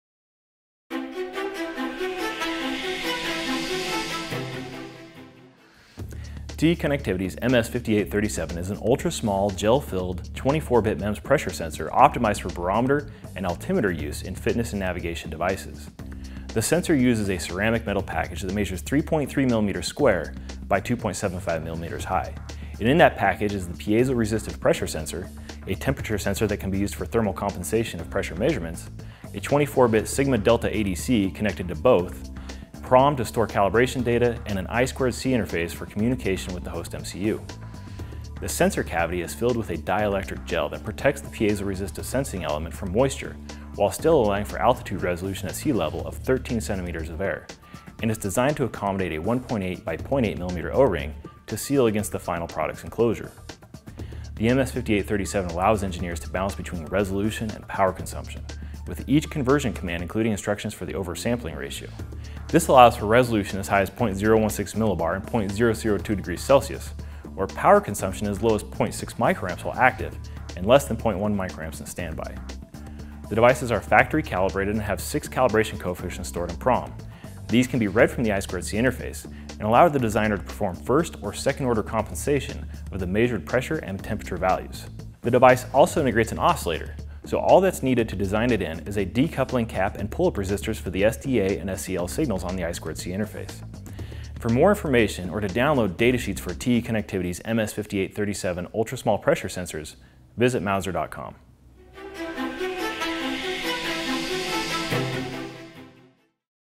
TE Connectivity's MS5837 | Featured Product Spotlight
TE Connectivity's MS5837 is an ultra-small, gel-filled, 24-bit MEMS pressure sensor optimized for barometer and altimeter use in fitness and navigation devices.

The sensor uses a ceramic metal package that measures 3.3 millimeters square by 2.75 millimeters high, and in that package is the piezoresistive pressure sensor, a temperature sensor that can be used for thermal compensation of pressure measurements, a 24-bit sigma-delta ADC connected to both, PROM to store calibration data, and an I2C interface for communication with the host MCU.

The sensor cavity is filled with a dielectric gel that protects the piezoresistive sensing element from moisture while still allowing for altitude resolution at sea level of 13 centimeters of air, and it’s designed to accommodate a 1.8 x 0.8 mm o-ring to seal against the final product’s enclosure.

Don't have AAC account yet? Register here for free, and always will be.